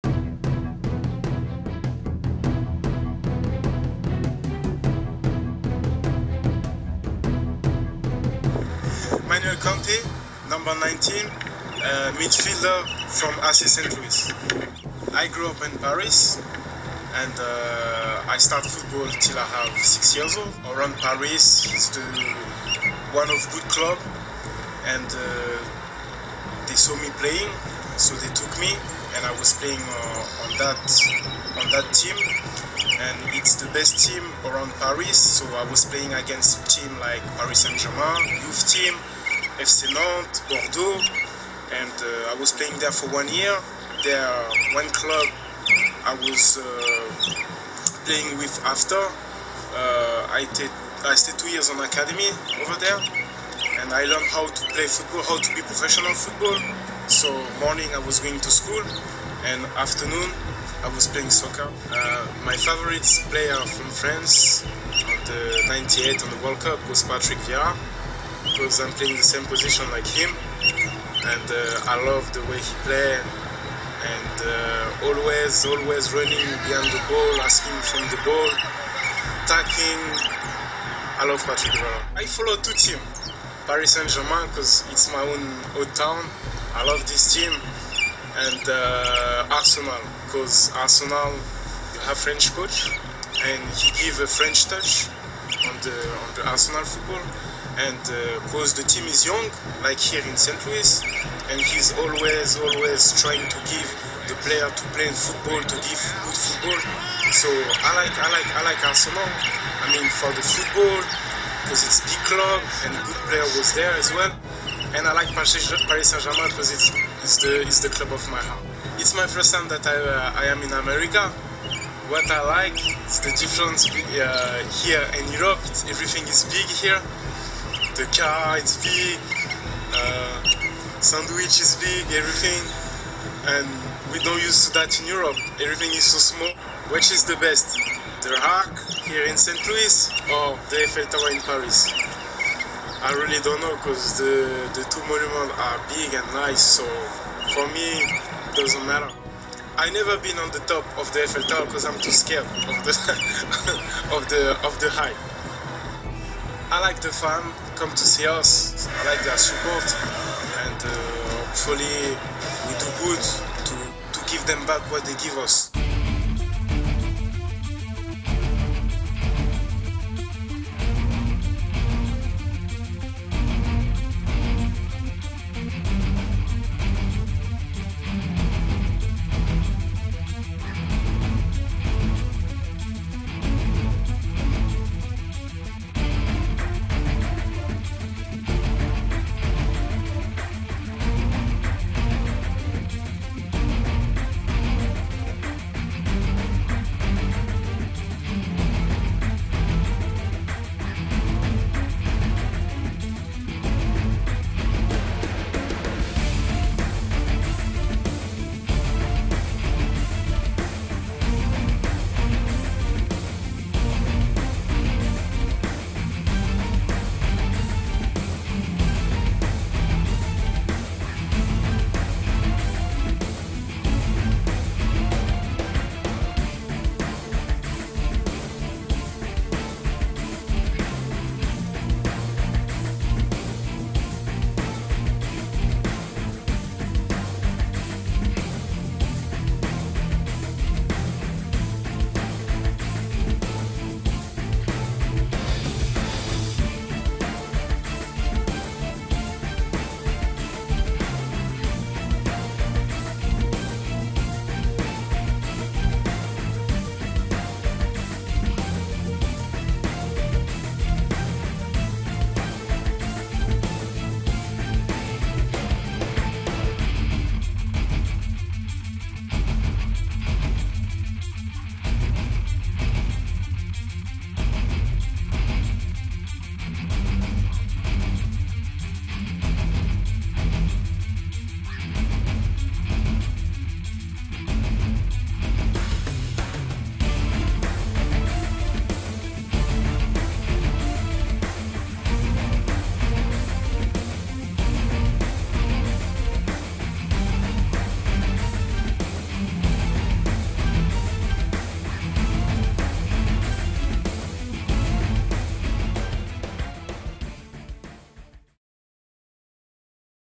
Manuel Kante highlights 1.mov
Manuel Kante interview and highlight video from games with AC st. Louis.